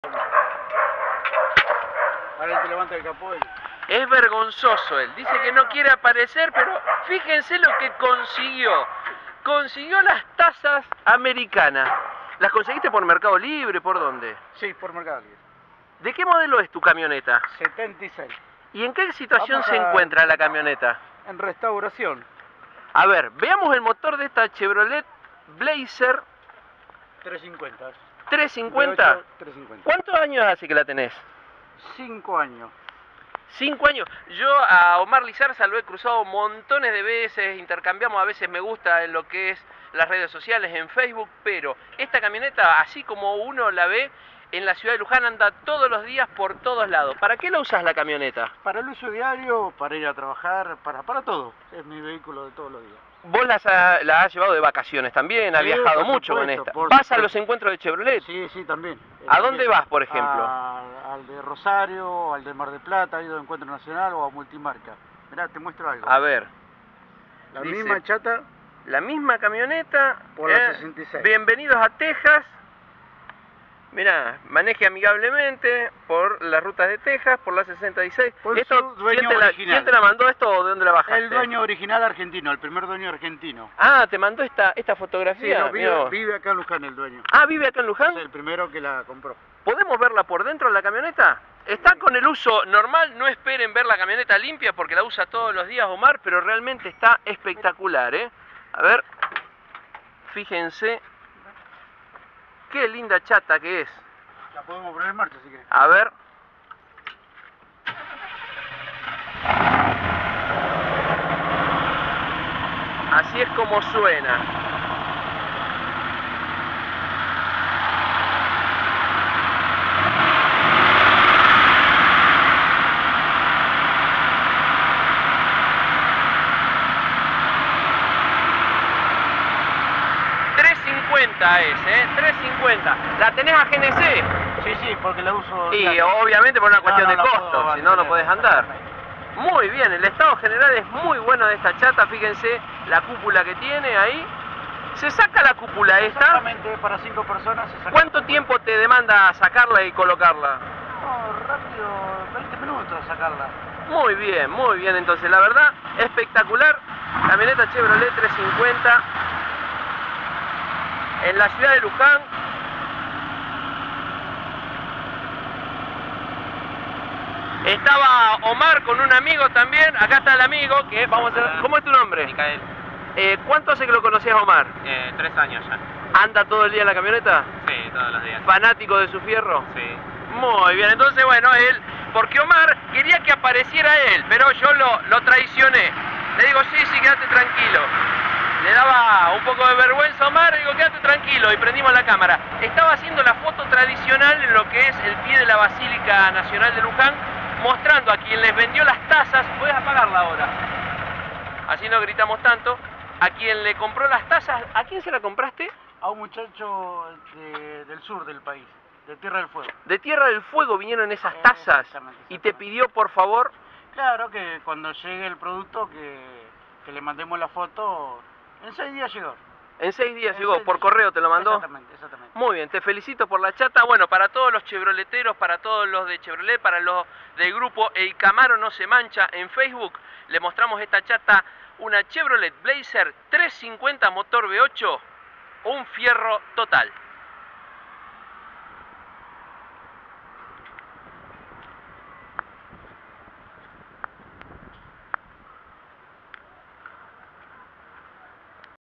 CHEVROLET La Blazer americana de Omar Lizarsa con motor Chevrolet V8 350
Omar Lizarza es vecino de la ciudad de Luján, participante de encuentros de fanáticos de la marca Chevrolet en distintas ciudades y provincias, y utiliza su camioneta Blazaer americana todos los días para sus actividades laborales, disfrutando de este vehículo clásico en cada jornada.
Lo encontramos de casualidad, cerca de la Basílica Nacional de Luján haciendo la tradicional fotografía que sacan muchos argentinos, y el motivo era mostrarle por las redes sociales cómo quedaban las tazas originales a quien se las había vendido. Estas tazas fueron enviadas desde Tierra del Fuego, y tardaron seis días en llegar a Luján.